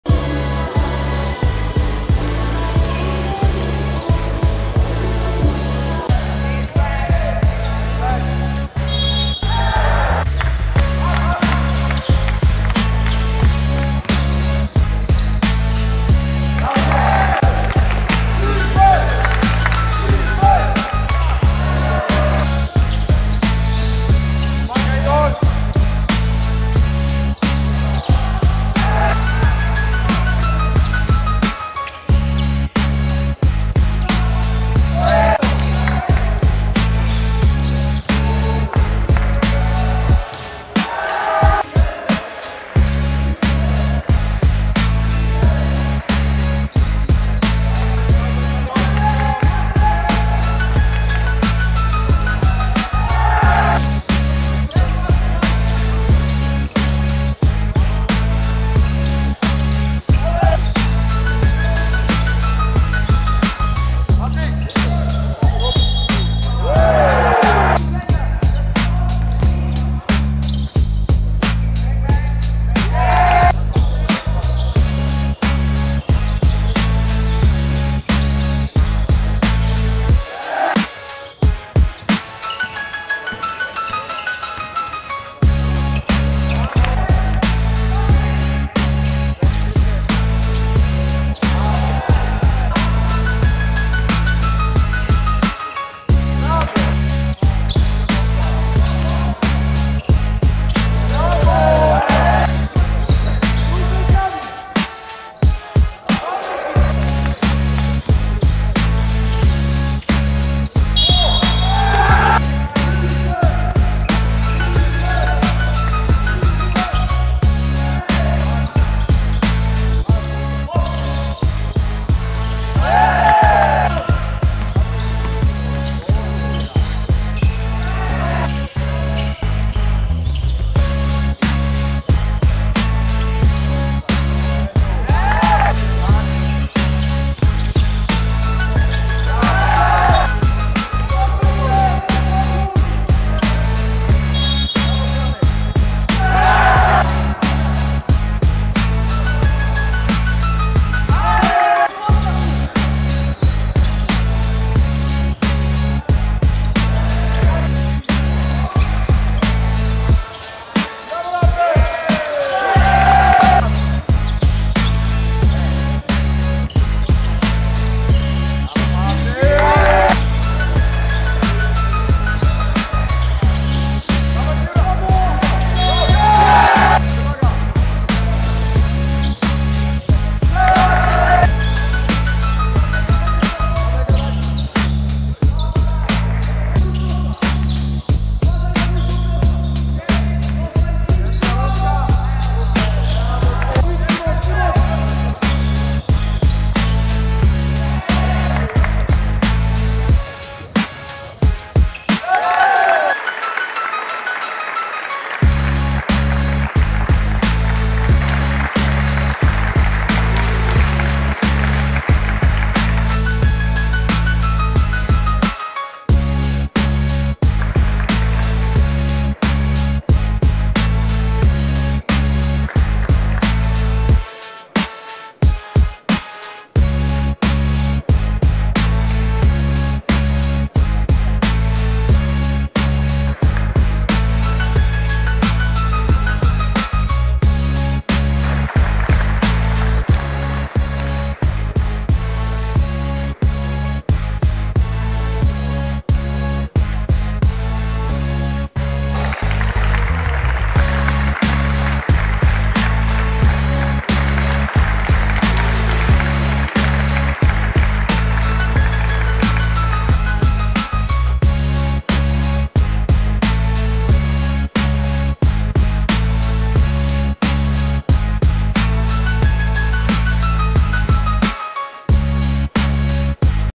24 07 FINAŁ Dallas vs Minnesota – Draft Camp
DALLAS 51:35 MINNESOTA
K.Stokłosa 16pkt 3ast
I.Janas 3pkt 8zb 5ast